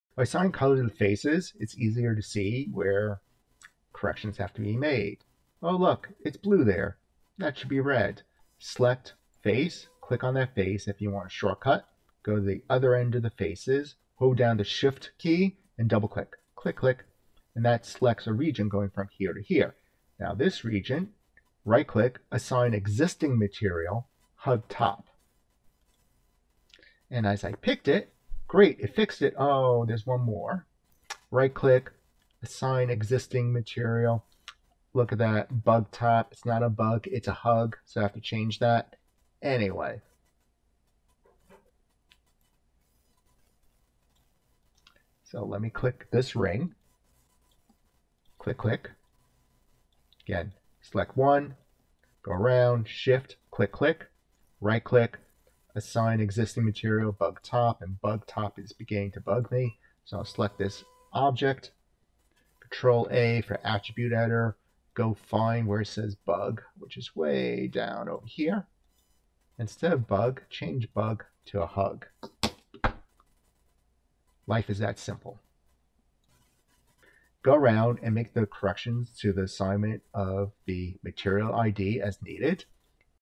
Maya and Assigning Material IDs to Selected Faces
Assign Material IDs on the face level to take a one-piece model and break it into multiple paint areas in Substance (or Unity, etc).